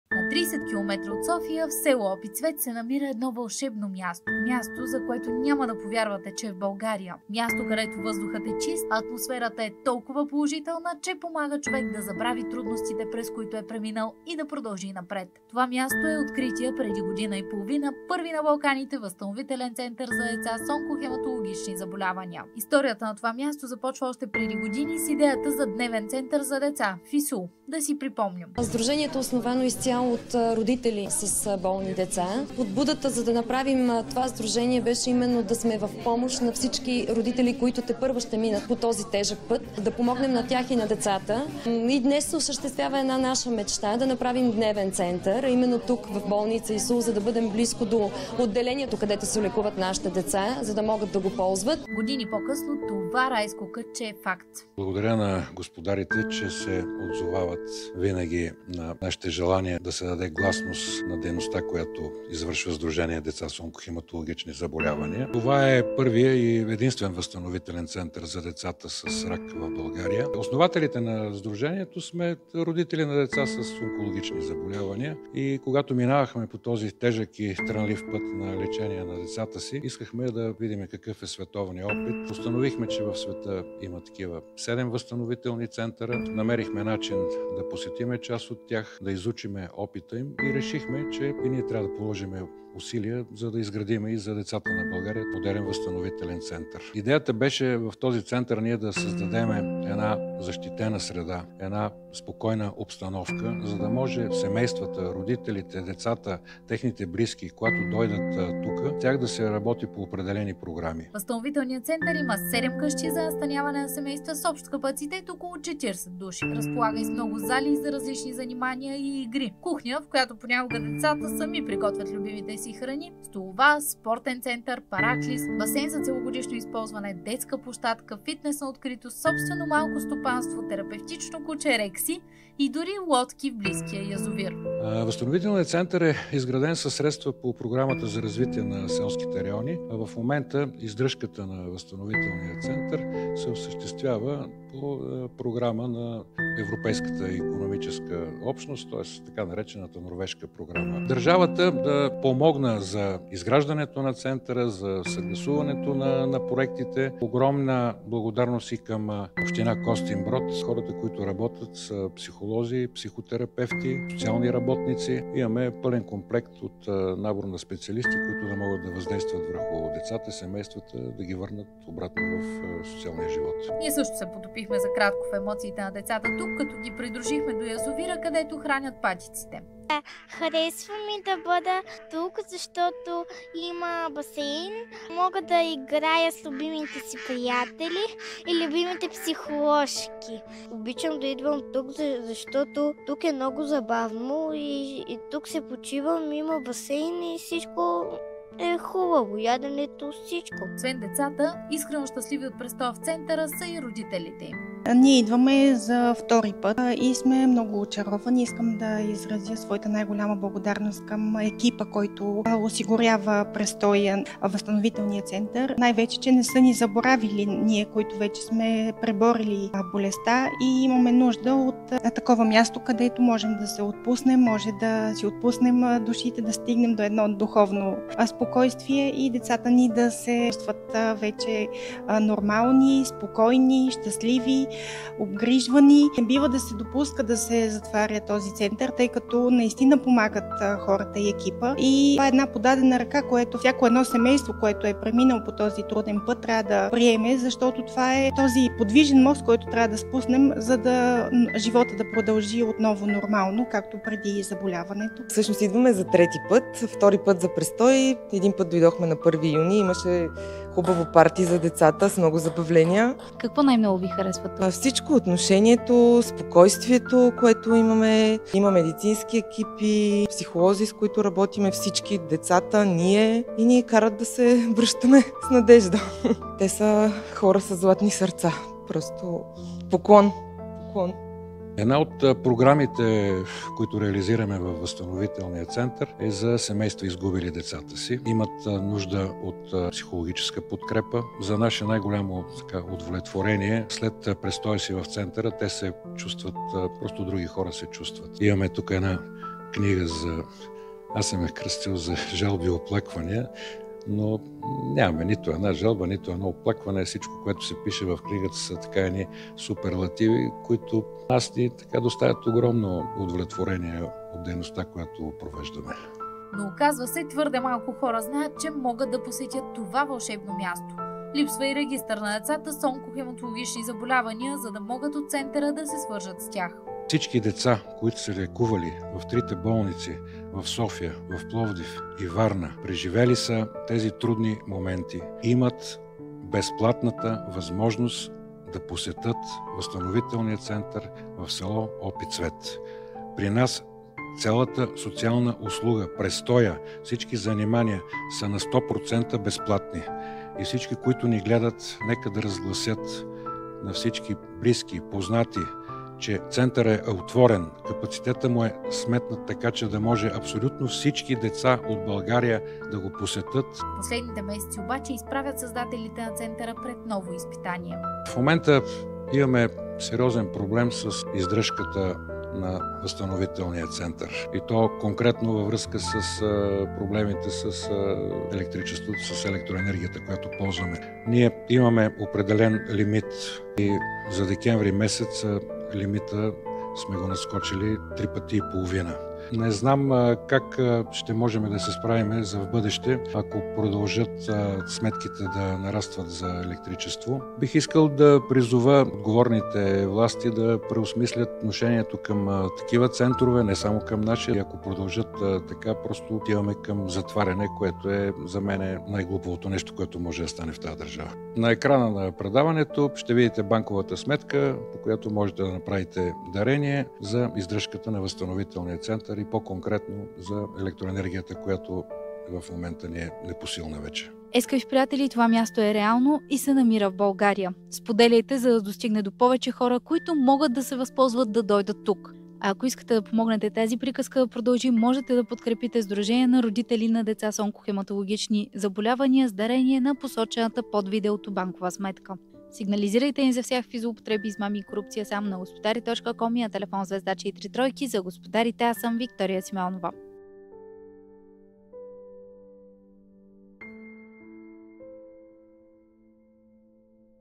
ВИЖТЕ ПЪРВИЯ ВЪЗСТАНОВИТЕЛЕН ЦЕНТЪР ЗА ДЕЦА С ОНКОЛОГИЧНИ ЗАБОЛЯВАНИЯ
Има места, за които не можем да повярваме, че са в България. Има каузи, които просто си заслужава да бъдат подкрепени. Повече за тази вижте във видеото!
Можете да подкрепите центъра с дарение на:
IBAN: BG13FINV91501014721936
Първа инвестиционна банка
Сдружение "Деца с онкохематологични заболявания"